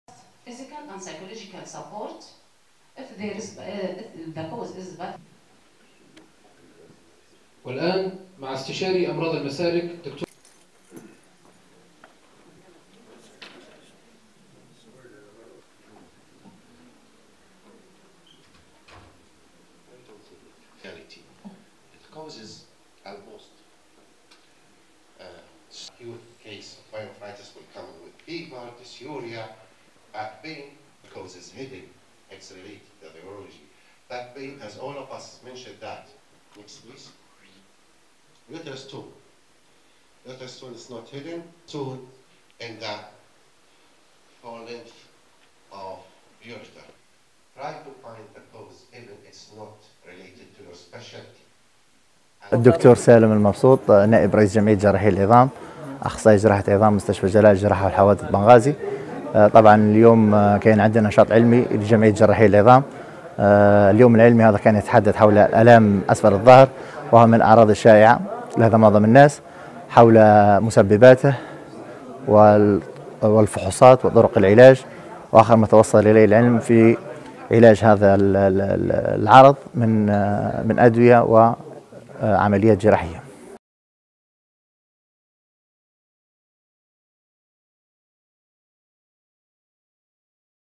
ورشة عمل في بنغازي حول تخصص جراحة العظام LCNA
وكالة الغيمة الليبية للأخبار - بنغازي.
نظم مستشفى الجلاء للحوادث والحروق ورشة عمل بعنوان (جراحة العظام في منطقة أسفل الظهر) والتي تعد شائعة بين الناس والذي يعد تهالك الطرق العامة والمطبات من أهم عوامل الإصابة بآلام أسفل الظهر.